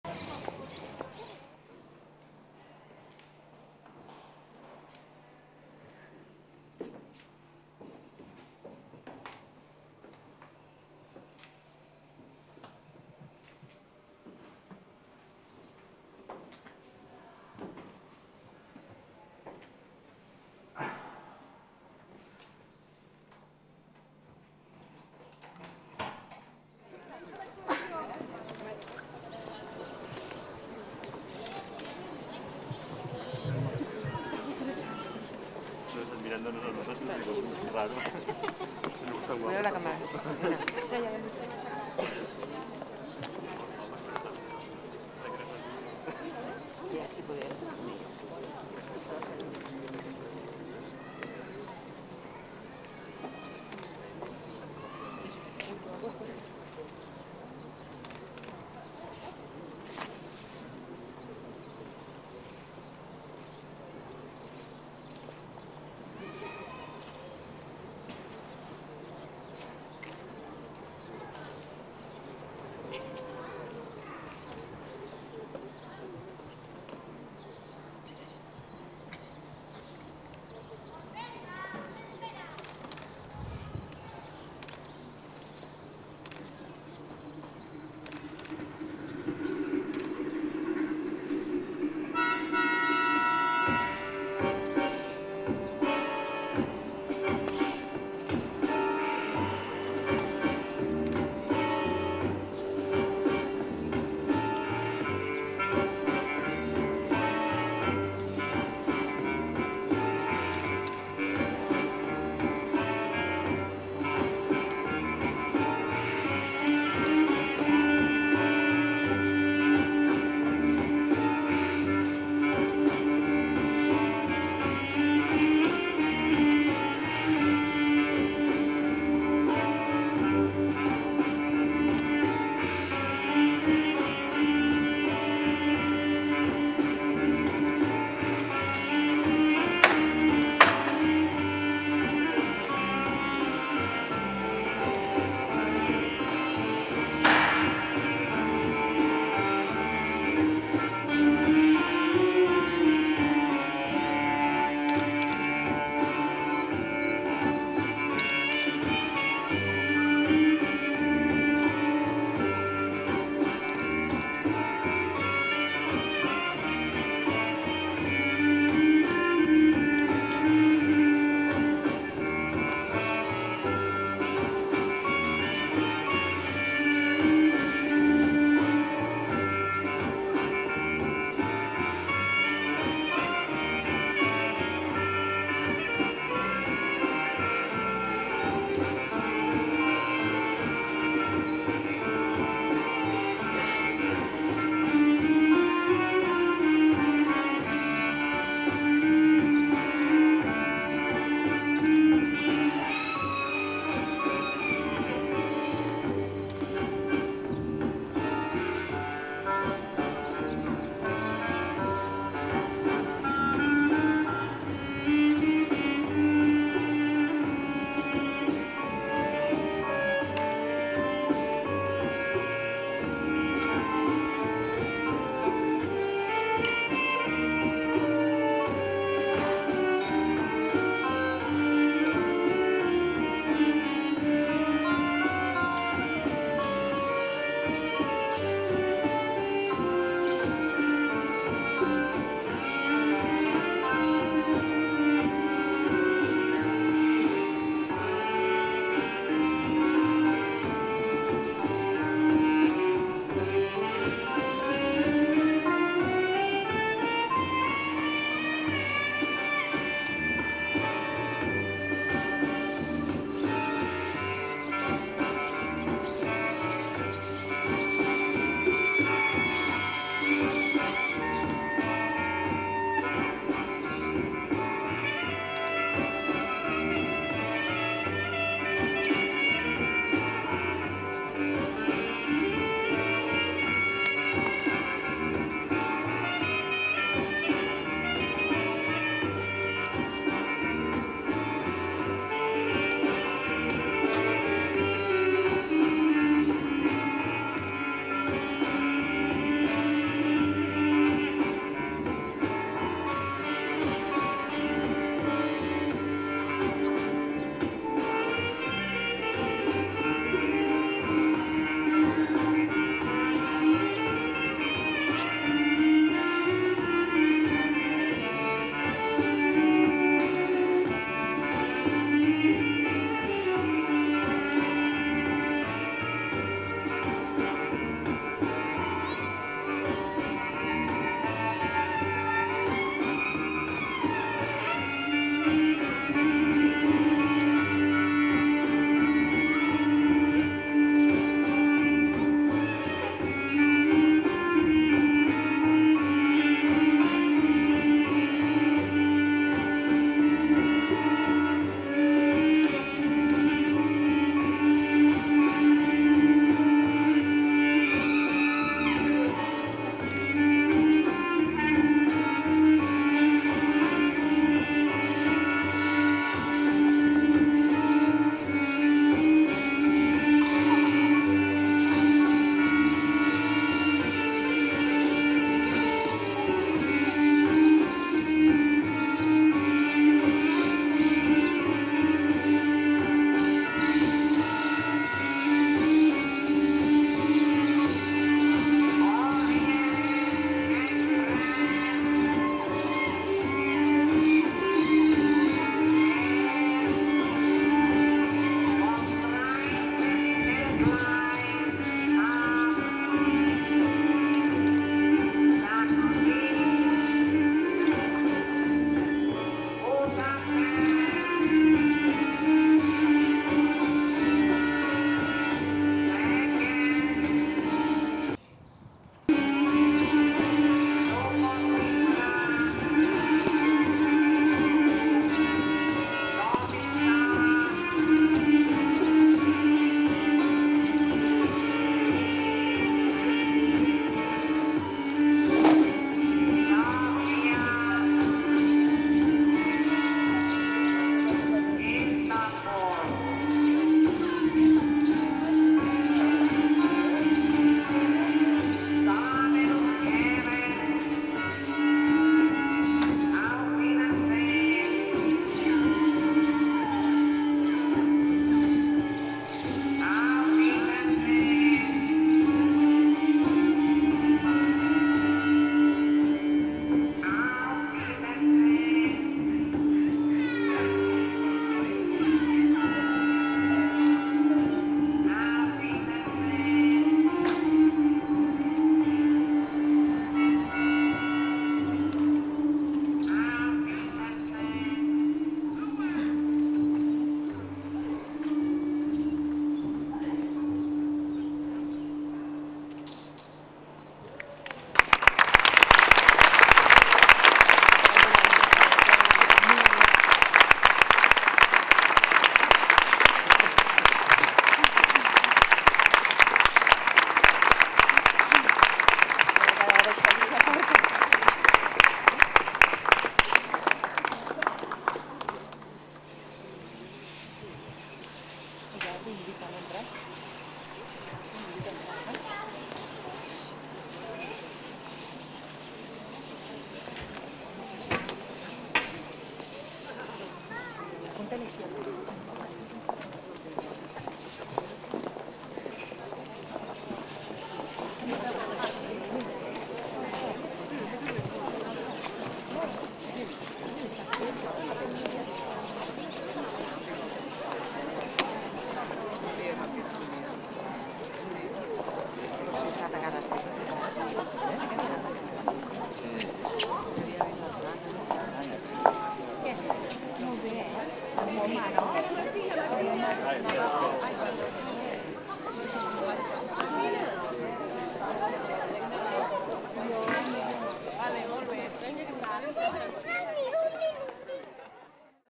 intervenció Torre de la Creu
propuesta de tragantDansa en edificio modernista